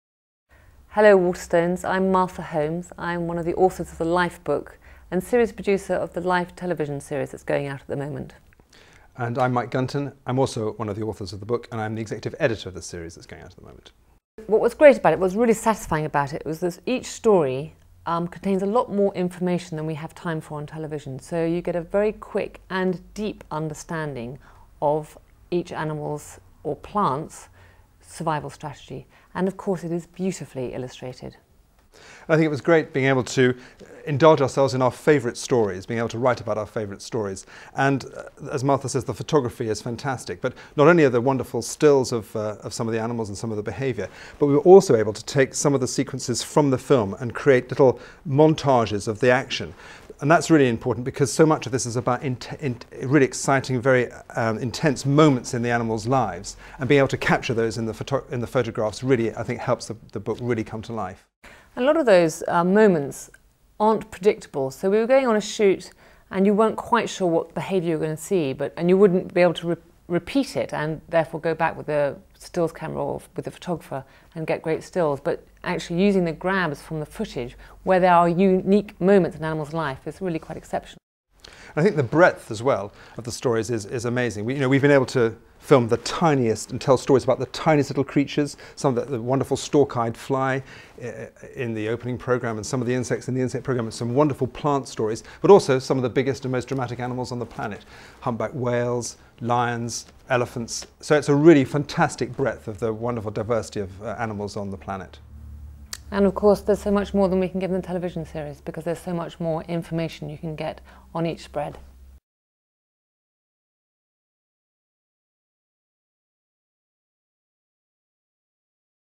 Life - the book and TV series 2 (narrated by David Attenborough) - Waterstone's
The producer and editor of the LIfe TV series, and the co-authors of the book talk about the making of the series and the content of the book. "Life" is an ambitious, exhilarating and beautifully illustrated study of animal behaviour, exploring the key groups of animals that inhabit this planet and their survival strategies. From cold-blooded reptilian killers to the armoured invaders of the insect world, the long-distance migratory birds to predatory mammals and the sociable and intelligent primates, "Life" reveals animal behaviour as never described before. By telling a series of unforgettable stories and focusing on intimate details, the book draws us into the biggest story of all - the many different ways animals and plants cope with the challenges of life and, ultimately, the survival of their species.